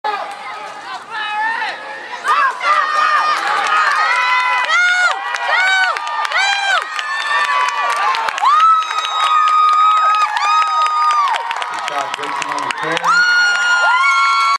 VTS_05_1.VOB
50yd run at smu Dallas Tx. Rashad Dixon 9yrs. old